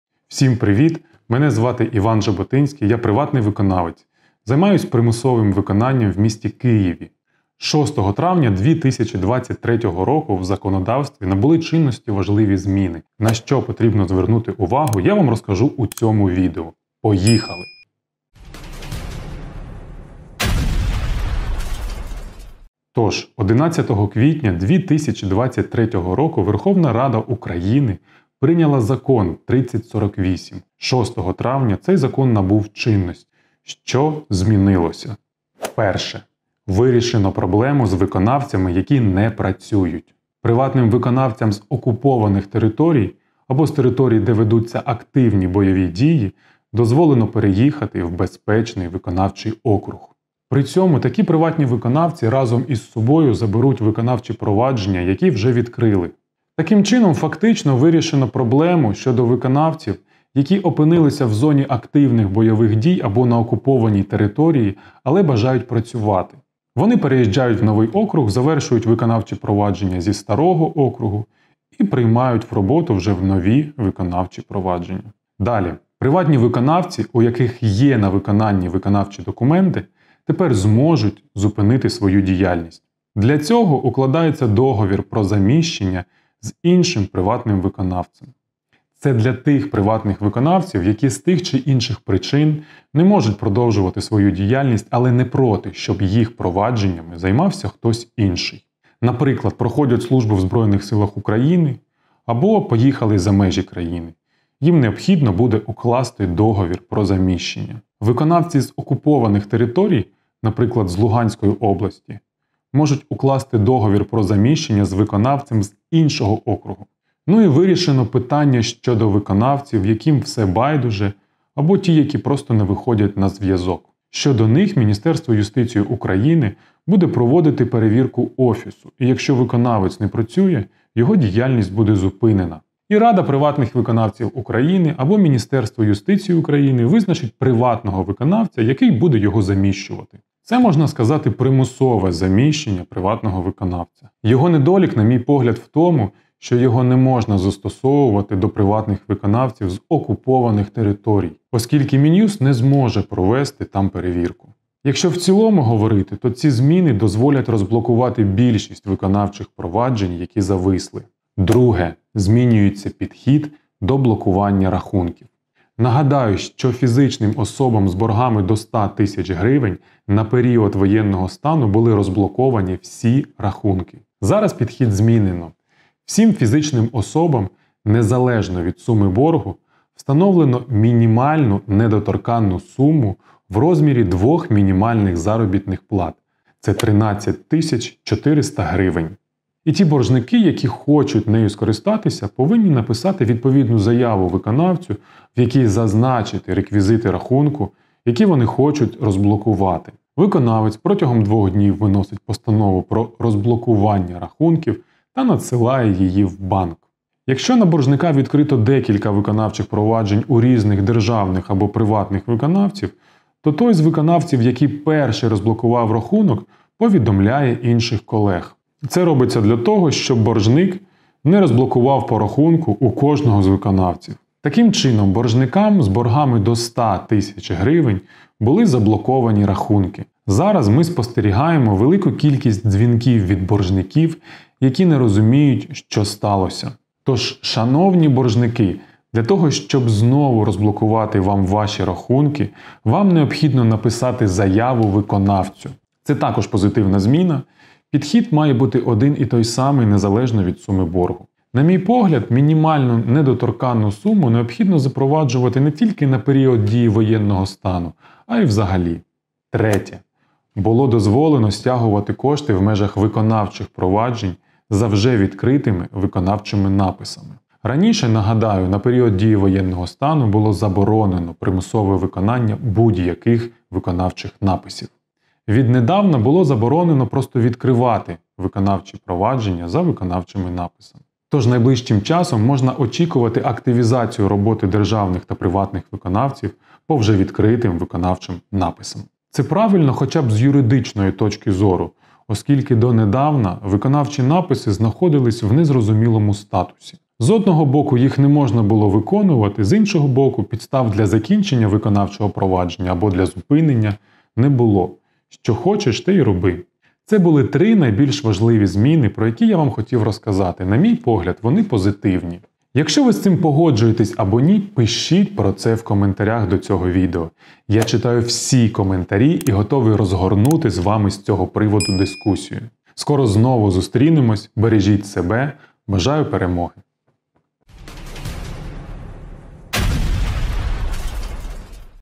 АРЕШТ РАХУНКІВ ТА ІНШІ ЗМІНИ У ВИКОНАВЧОМУ ПРОВАДЖЕННІ З 06.05.2023. АНАЛІЗ ЗАКОНУ УКРАЇНИ № 3048-ІХ
Всім привіт, мене звати Іван Жаботинський, я приватний виконавець. Займаюсь примусовим виконанням в місті Києві. 06 травня 2023 року в законодавстві набули чинності важливі. В цьому відео я розкажу на що варто звернути увагу. Поїхали.

Тож, 11 квітня 2023 року Верховна Рада прийняла № 3048-ІХ, який набуває чинності 06. травня. Що змінилося.

Перше. Вирішено проблему з виконавцями, які не працюють.

Приватним виконавцям з окупованих територій, або територій, де ведуться активні бойові дій дозволено переїхати в безпечений округу. При цьому такі викоанвця разом із собою заберуть виконавчі провадження, які відкриті до цього.

Фактично вирішено проблему для виконавців, які опинилися в зоні бойових дій, або на окупованій території, але бажають працювати. Вони переїжджають в новий округ, там вже доводять до завершення провадження з попереднього округу і приймають в роботу документ в новому окрузі.

Приватні виконавці, у яких в роботі є виконавчі документи можуть зупинити свою діяльність. Для цього укладається договір про заміщення з іншим приватним виконавцем.

Це для тих виконавців, які з тих чи інших причин не можуть працювати, але не проти віддати сої провадження іншим. Наприклад проходять службу в лавах Збройних сил, або за межами україни. Вони просто укладають договір заміщення з іншим виконавцем. Виконавці з окупованих регіонів можуть укласти договір про заміщення з кимось з іншого виконавчого округу.

Приватні виконавці, яким все байдуже, і які не виходять на зв'язок. Щодо них Мін'юст буде проводити перевірку офісу. Якщо виконавця в офісі немає, йому зупиняється діяльність і РПВУ, або Мінюст визначає приватного виконавця, який його заміщує.

Це можна сказати примусове заміщення, для приватних виконавців. Його недолік на мій погляд в тому, що його не можна застосовувати до приватних виконавців з окупованих територій. Мінст не зможе провести там перевірку.

Якщо в цілому говорити, що ці змінять повинні розблокувати більшість виконавчих проваджень, які зависли.

Друге. Змінюється підхід до блокування рахунків. Нагадаю, що на період дії воєнного стану було дозволено фіз. особам вільно користуватися коштами маючи борги до 100 тис. грн. Зараз підхід змінюється.

Для всіх боржників фізичних осіб незалежно від суми боргу встановлюється мінімальна недоторкана сума в розмірі 2 мзп. - це 13 400 грн. Ті боржники, які хочуть нею скористатися повинні написати заяву виконавцю, в якій зазначити реквізити рахунку, що вони хочуть розблокувати. Виконавець протягом двох днів виносить постанову про розблокування та надсилає її в банк. Якщо виконавче провадження відкрито у декількох виконавців, то перший, хто розблокував рахунок боржника повідомляє про це інших державних, або приватних виконавців. Це щоб не було так, що боржник у кожного викоанвця розблокував по рахунку.

Таким чином, боржникам з боргами до 100 тис. грн. були заблоковані рахунки. Зараз ми спостерігаємо багато дзвінків від здивованих боржників, які не розуміють чому їм заблокували банківські карти.  Тож, шановні боржника, для того, щоб їх знову розблокувати потрібно написати заяву виконавцю.

В цілому це позитивна зміна. Підхід повинен бути однаковим незалежно від суми боргу. На мій погляд недоторкану суму потрібно запроваджувати не тільки на період дії воєнного стану, і взагалі.

Третє. Було дозволено стягувати кошти в межах вже відкритих виконавчих проваджень за виконавчими написами нотаріусів. Раніше на період дії воєнного стану забороняється будь-яке примусове виконання виконавчих написів. Зараз просто забороняється відкриття виконавчих проваджень за виконавчими написами.

Можна очікувати активізацію виконавців, про вже відкритим виконавчим написам.

В цілому це правильно. Ну хоча б з юридичної точки зору. Оскільки раніше виконавчі написи знаходяться в незрозумілому статусі. З одного боку їх не можна було виконувати, з іншого підстав для закриття або зупинення провадження також не було. Що хочеш, те і роби.